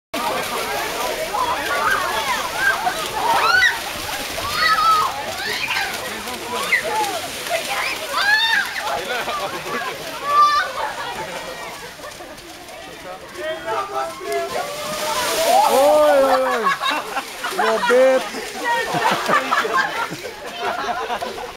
Kalvruset 2009 - badande kille / runner swimming...
One of the runners in Kalvruset 2009 encountered some difficulties when negotiating the creek that the runners had to pass after about 1 K into the race...His choice of clothes (cotton) was perhaps not the optimal one given the conditions...Listen to the shrieks when the kids realize that they actually have to pass the creek!